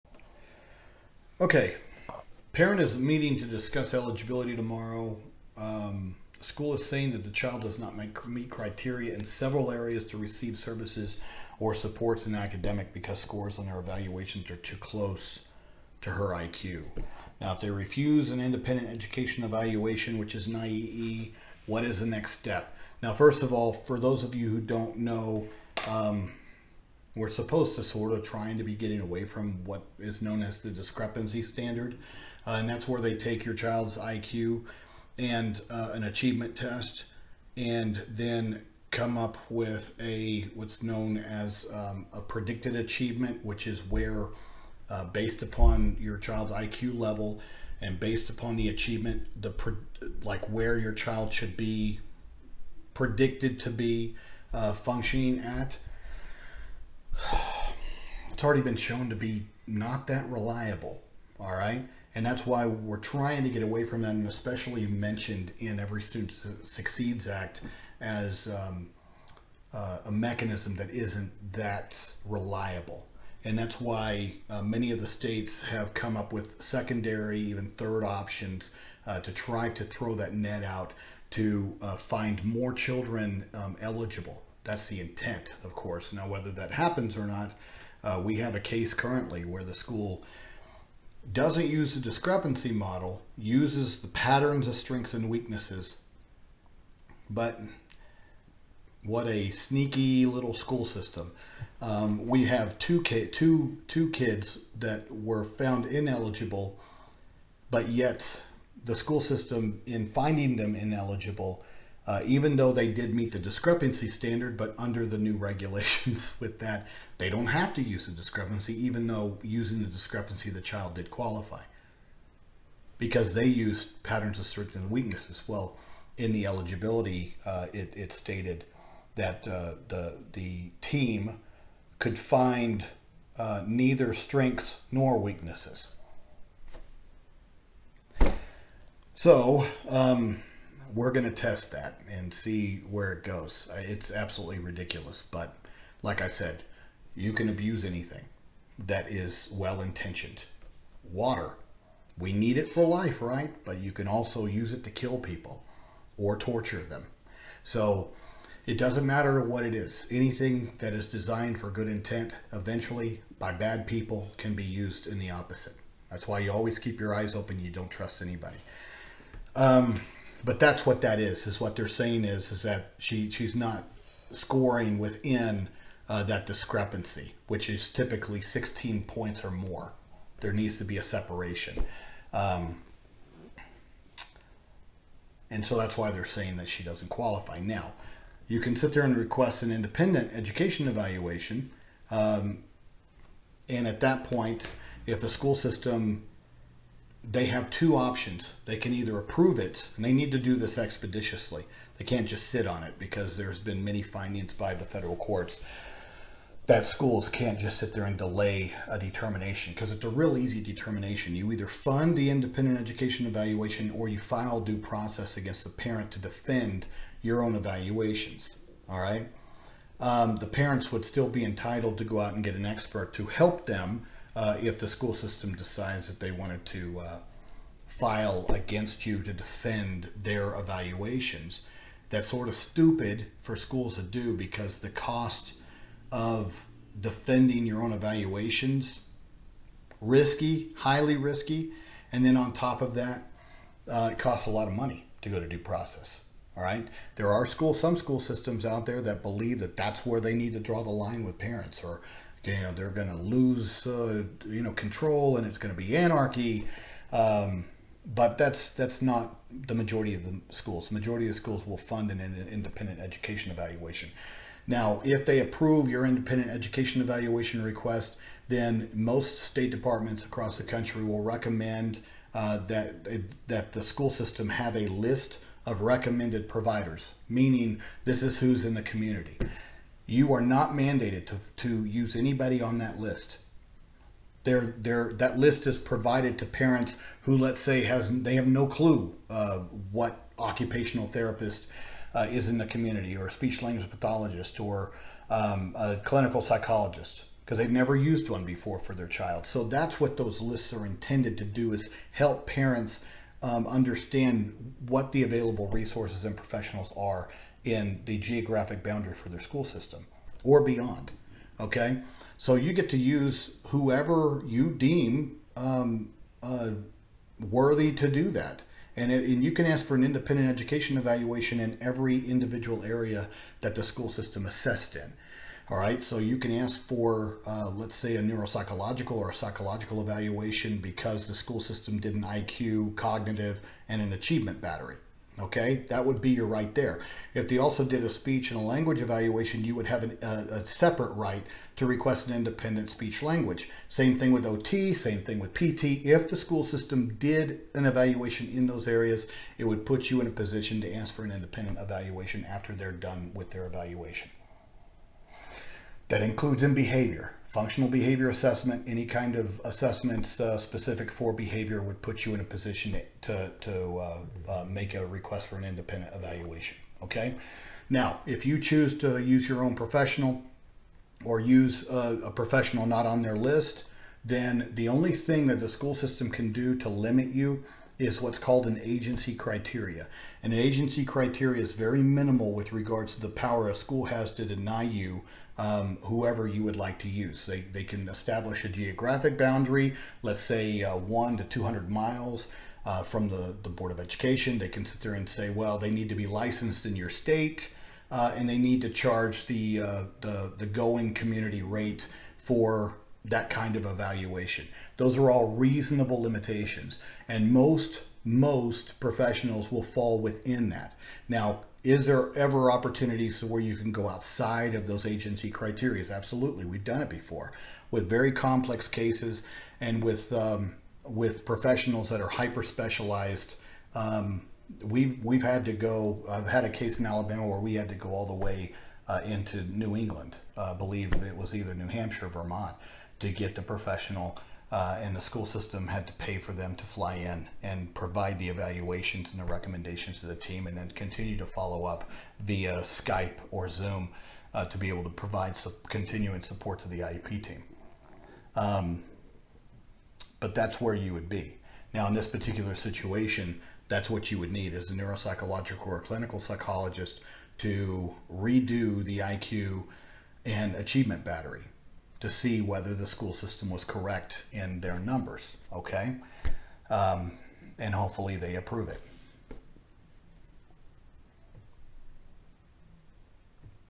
IQ/Achievement test and the discrepancy model and explaining the Independent Evaluation process
The Question Asked:
"A parent is meeting to discuss eligibility tomorrow. The school is saying the child does not meet criteria in several areas to receive services or support in academics because scores on their evaluation are too close to her IQ. If they refuse an IEE, what is the next step?"